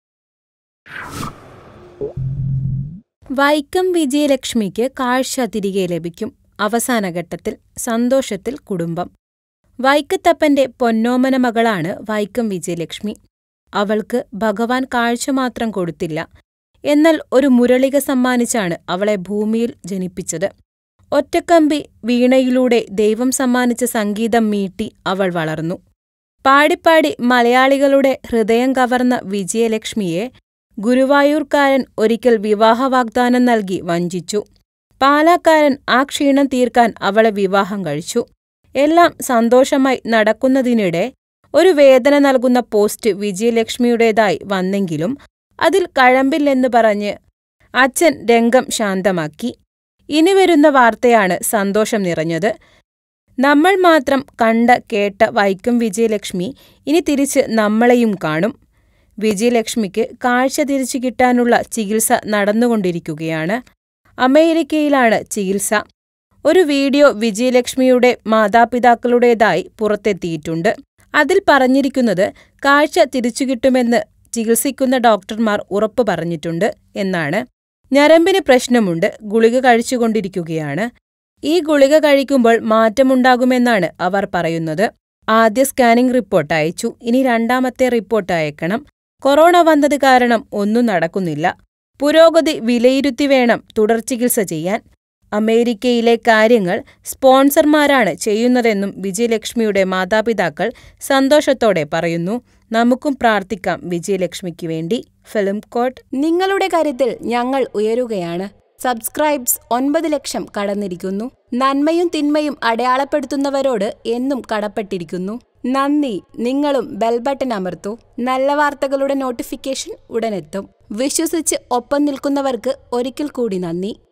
വൈക്കം വിജയലക്ഷ്മിക്ക് കാഴ്ചതിരികെ ലഭിക്കും അവസാനഘട്ടത്തിൽ സന്തോഷം കൊണ്ട് കുടുംബം
Watch വൈക്കം വിജയലക്ഷ്മിക്ക് കാഴ്ചതിരികെ ലഭിക്കും അവസാനഘട്ടത്തിൽ സന്തോഷം കൊണ്ട് കുടുംബം